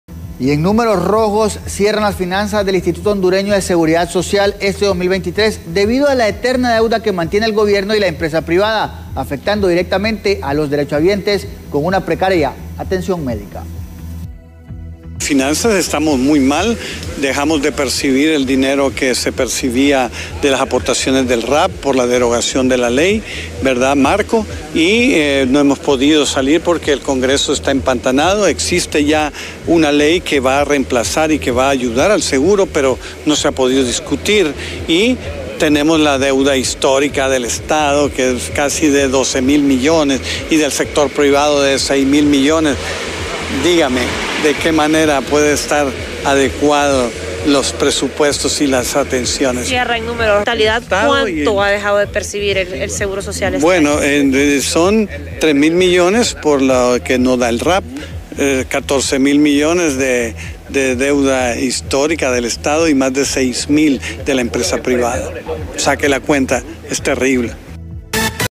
Más de 3 millones de lempiras en pérdidas reporta el IHSS
En números rojos cierran las finanzas el Instituto Hondureño de Seguridad Social este 2023 debido a la eterna deuda que mantiene el gobierno y la empresa privada.
Esto ha afectado directamente a los derechohabientes con una precaria atención médica.